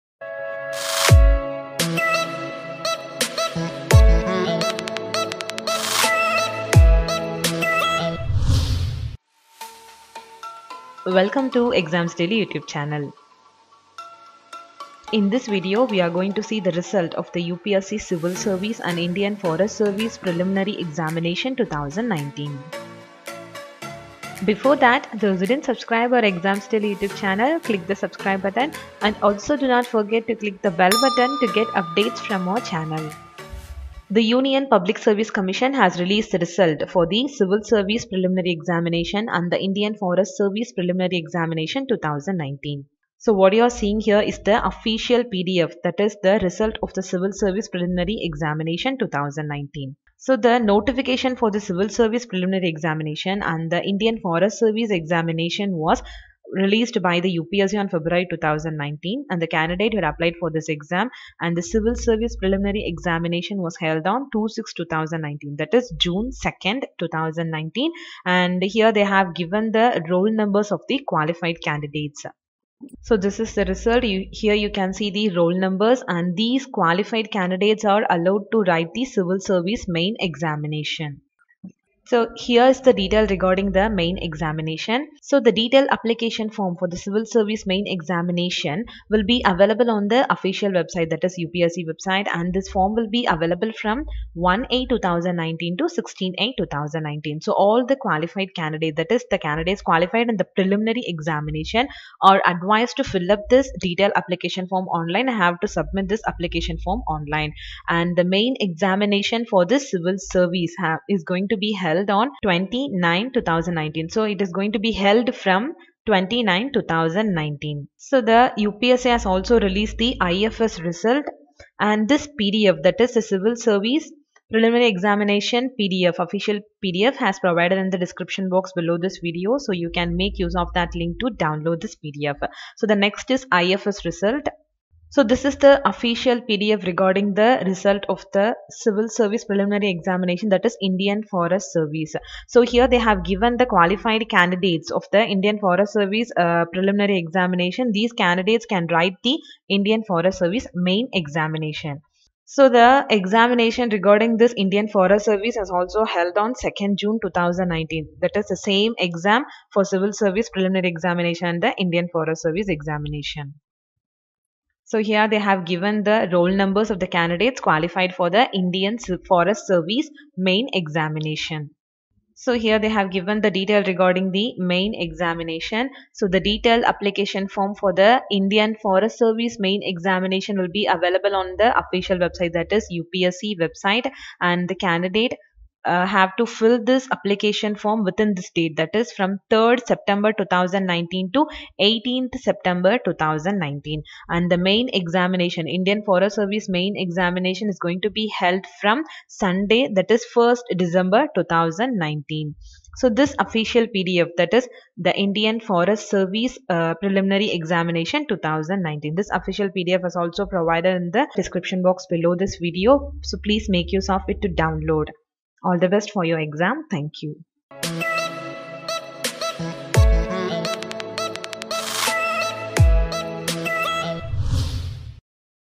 UPSC Civil Service Prelims Result 2019 I UPSC Indian Forest Service Prelims Result 2019
UPSC Civil Service 2019 Prelims Result /UPSC Indian Forest Service 2019 Prelims Result- Released

Union Public Service Commission (UPSC) has released Result for Civil Service Prelims Exam 2019. The Prelims Exam was held on June 2, 2019. Candidates can download the UPSC Civil Service 2019 Prelims Result from the following link.Union Public Service Commission (UPSC) has released Result For Indian Forest Service 2019 Prelims Examination.Preliminary Examination, 2019 was held on 2nd June, 2019. Selected Candidates are qualified for mains examination. Candidates can download the UPSC Indian Forest Service 2019 Prelims Result from the following link.